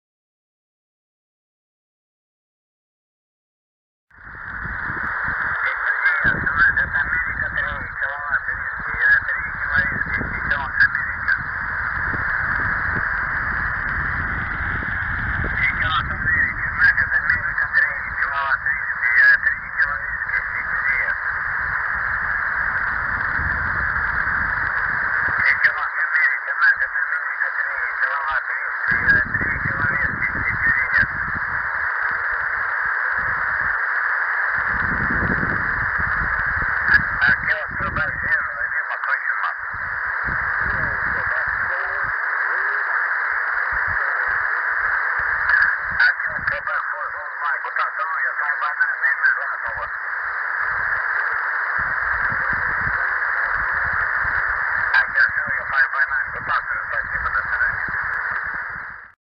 UA3KW received on GP-5/SSB (aka Tecsun PL-365)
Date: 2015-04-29
Time: ~03:23UTC
RX: CountyComm GP-5/SSB
QTH: Folsom Lake, California